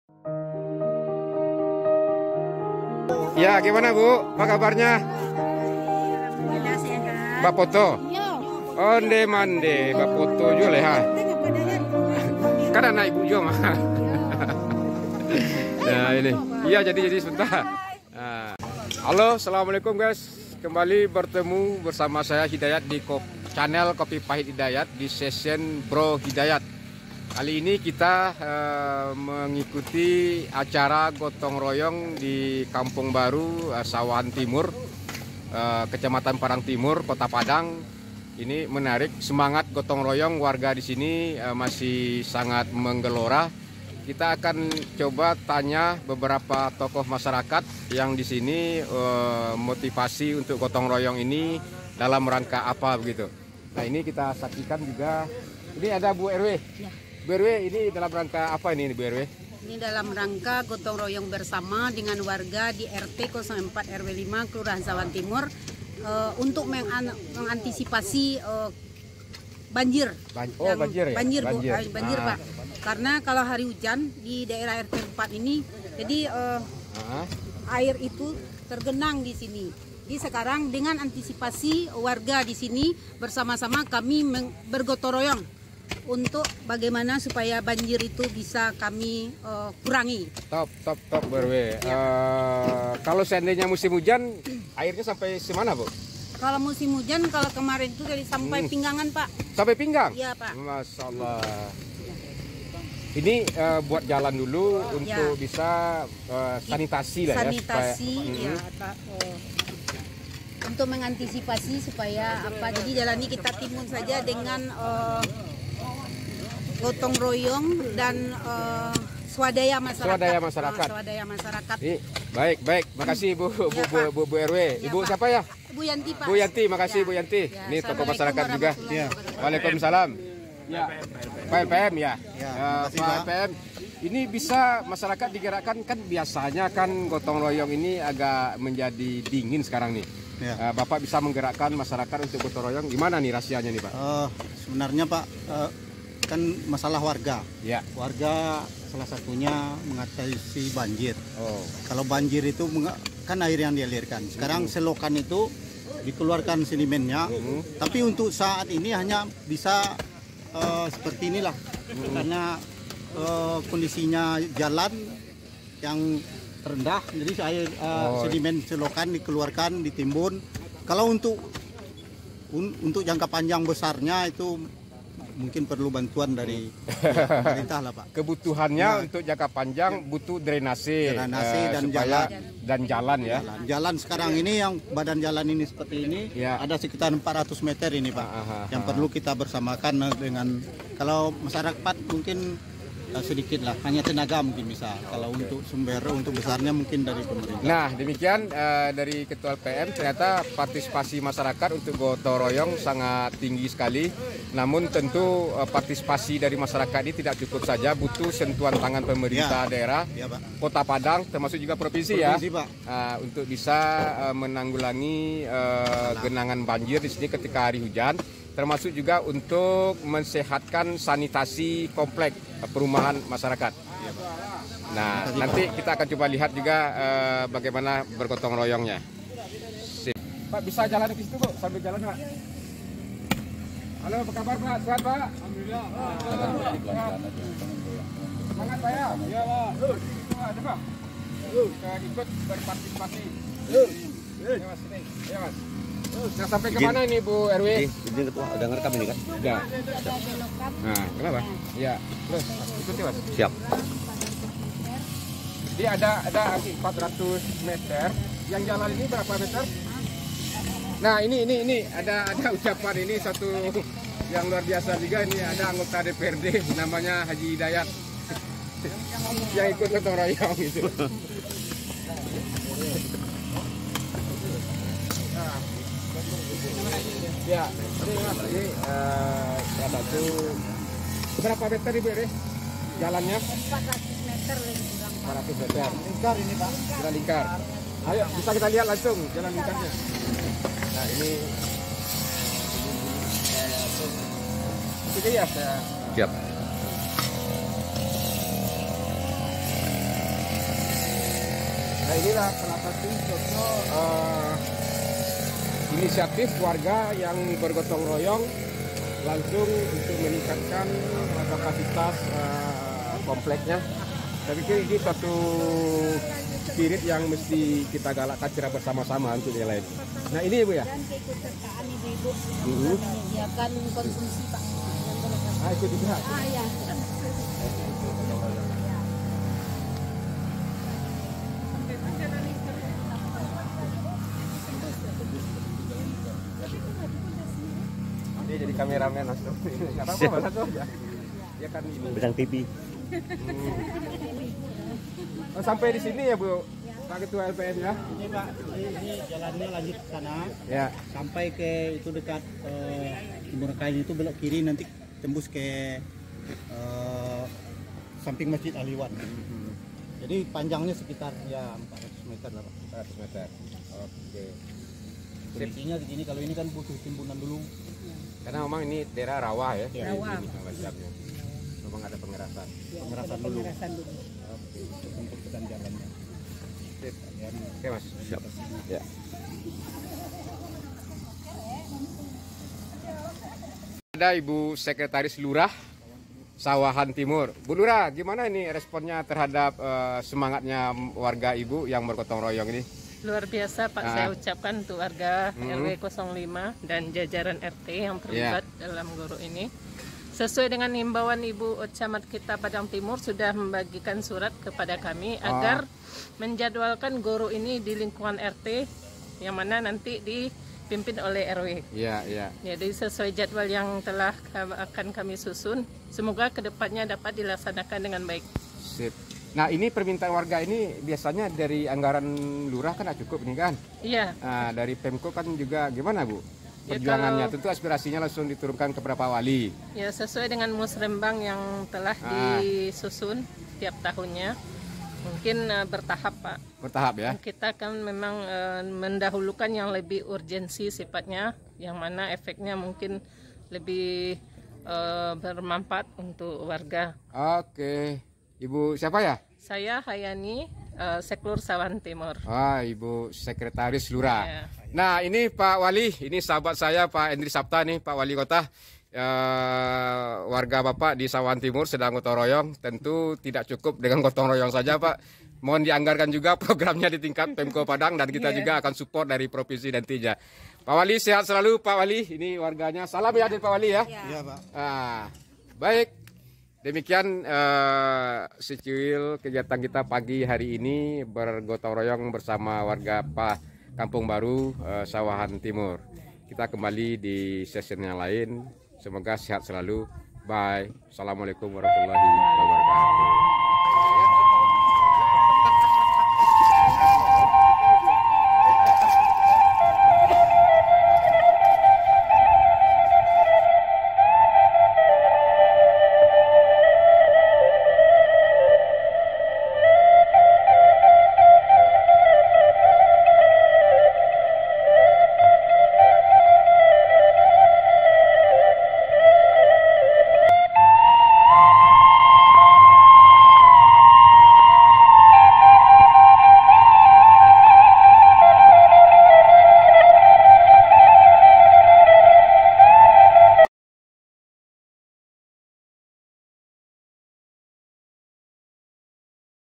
AYO KITA GORO...
Semangat gotongroyong warga Kampung Baru, Sawahan Timur Kota Padang. Selain sarana memperkuat silaturrahmi sesama warga, momen goro juga dimanfaatkan berbagi informasi terkait agar tidak terpapar virus corona yang diatasi dengan memperkuat imun kesehatandengan mengeluarkan keringat atau berjemur di sinar matahari

salam kompak Warga Kampung Baru